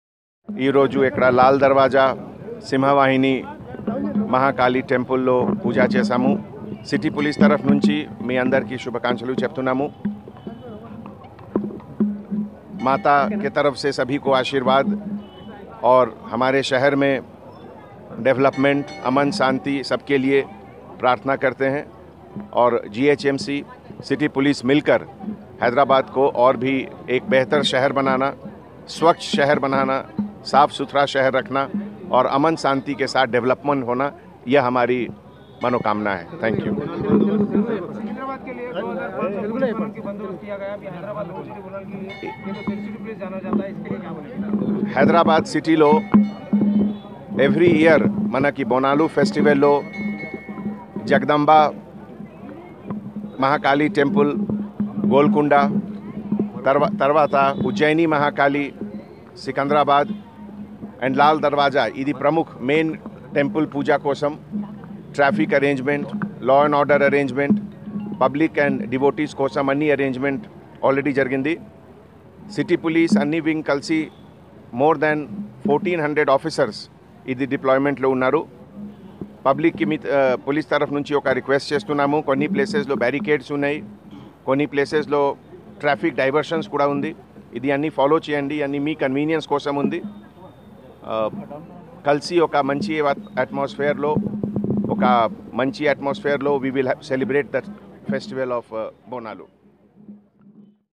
Commissioner of police Anjani Kumar visit Lal Darwaza Temple || Hyderabad...
REPORTER: MOHD MOMIN SHAREEF (Rizwan)